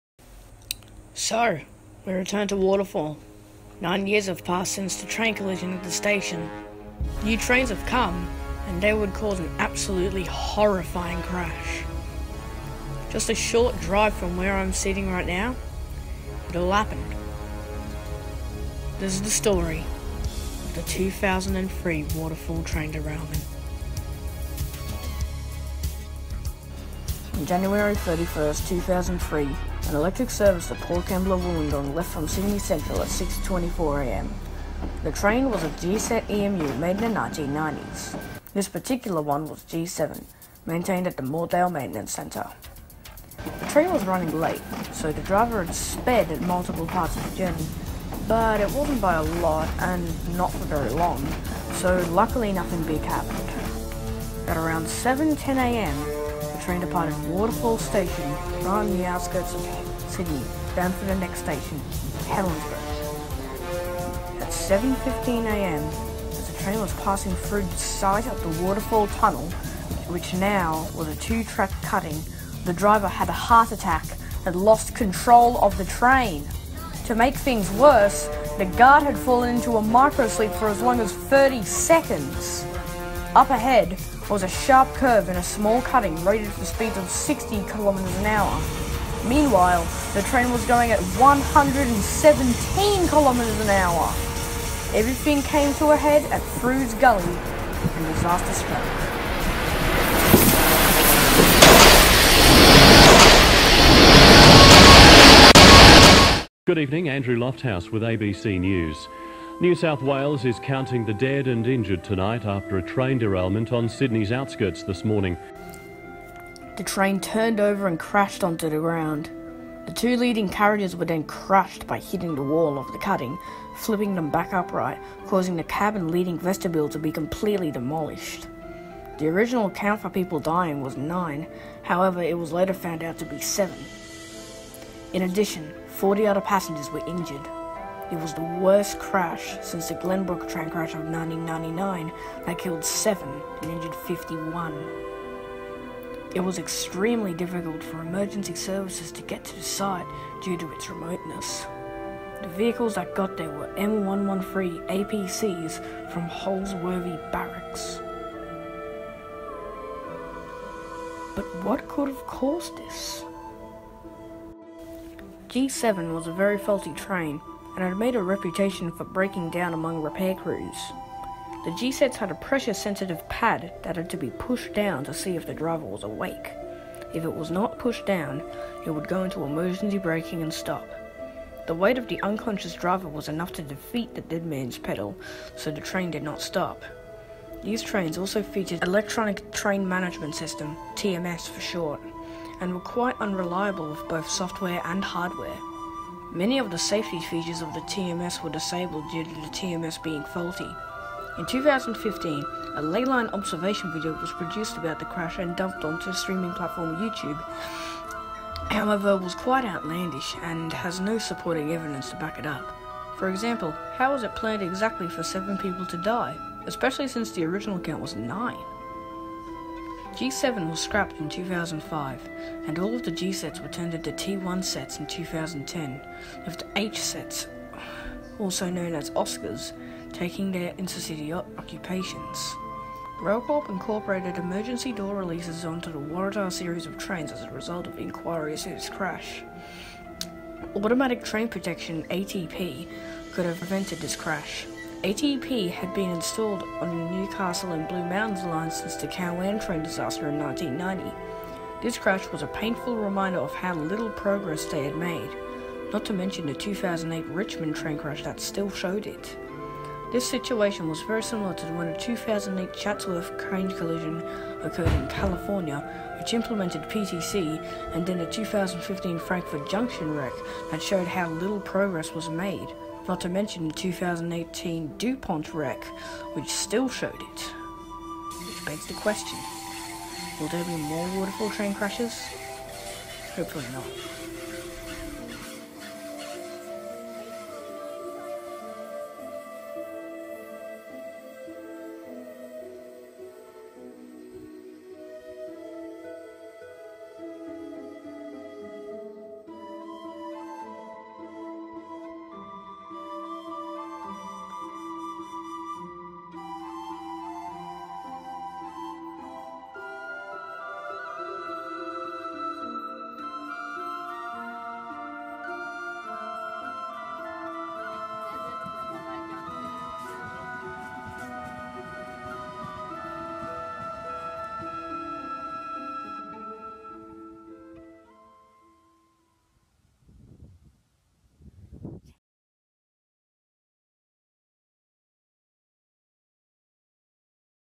2003 Waterfall Train Disaster 17 years later(400 Subscriber special Pt 1)
The second of the 3 final videos for a while. I'll be taking a break to focus on school in the new term.
The final part will be the 1969 Violet Town collision. Sorry if it's a bit late.
Also small goof I made, I said that it occured on the 25th when it was the 29th. Whoops!